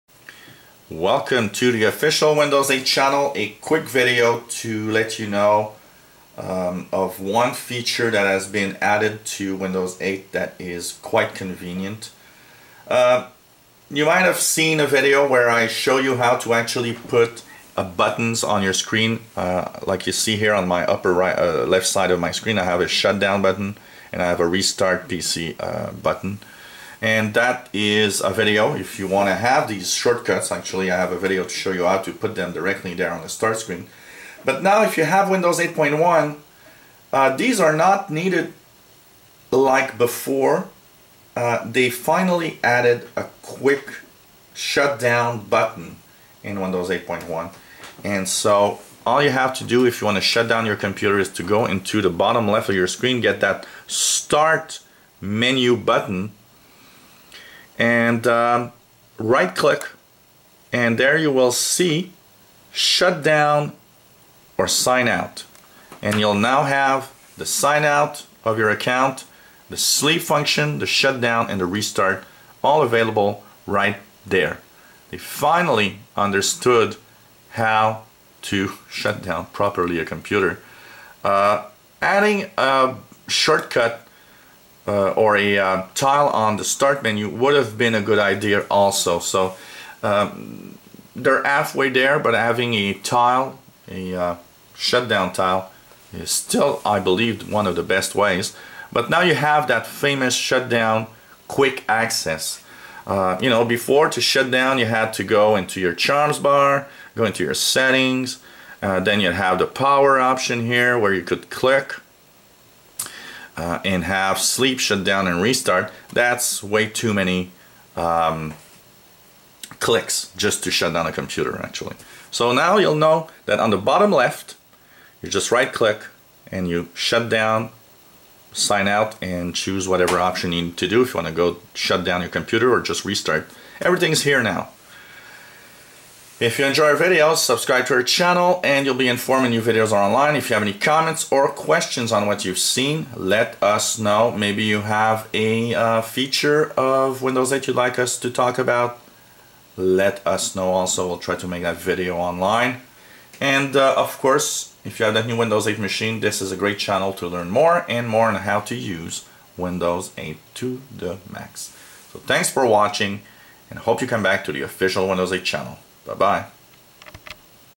Windows 8.1 new sleep shutdown and restart feature
New way to access sleep, shutdown and restart in Windows 8.1